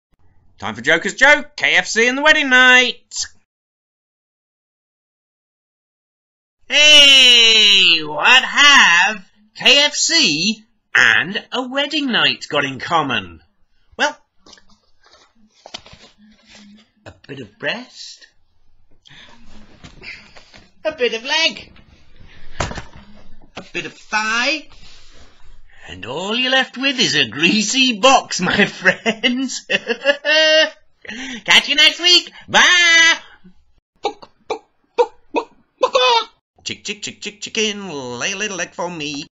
Joker's Joke - KFC & Wedding Night
The Joker is back with strangely another chicken related joke.
BOOK BOOK BOOK BookHURT